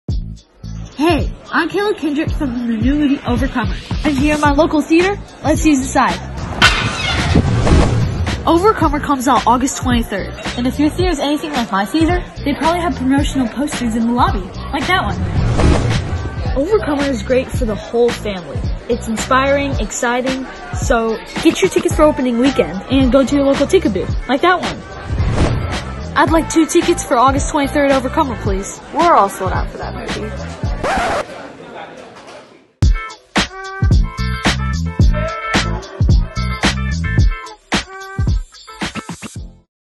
Overcomer Tickets
Caleb Kendrick getting tickets to see his film Overcomer.